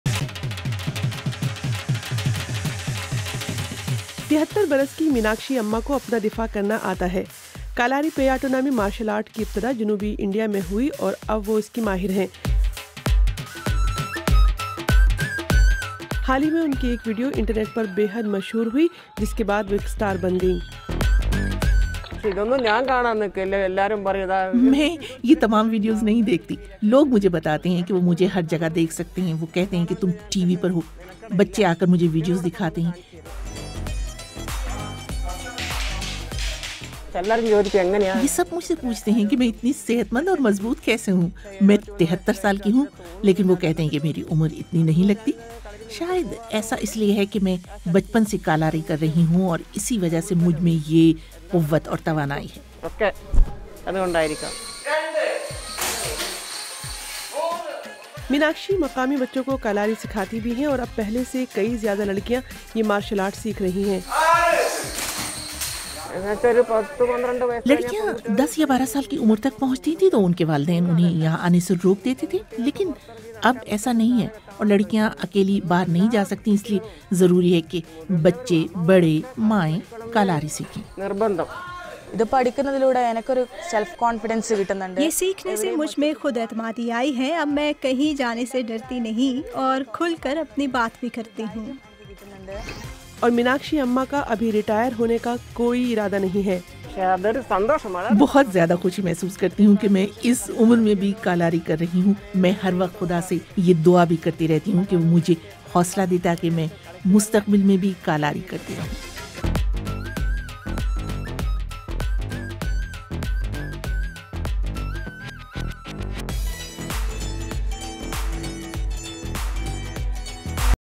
The sword-fighting granny from India.
A 73-year-old grandmother in Kerala in Southern India has become a social media star after appearing in a video in which she demonstrated her martial arts prowess.
Now Meenakshi Amma is passing on her skills to a younger generation, including increasing numbers of girls who are learning how to defend themselves.